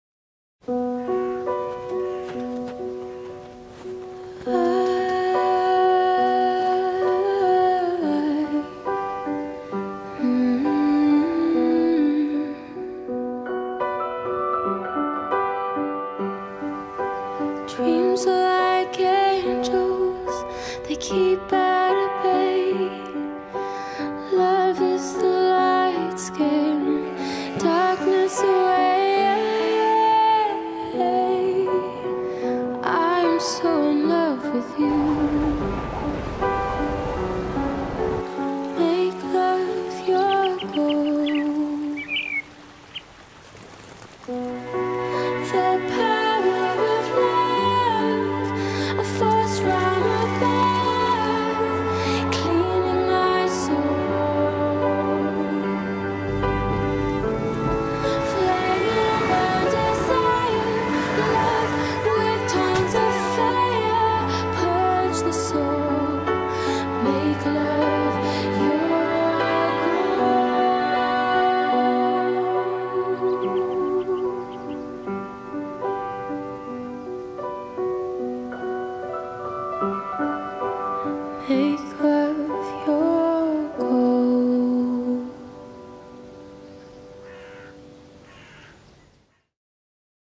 Publicité de Noël John Lewis (2012)
La toute dernière publicité de traditionnelle de Noël de la chaine de magasins anglaises " John Lewis".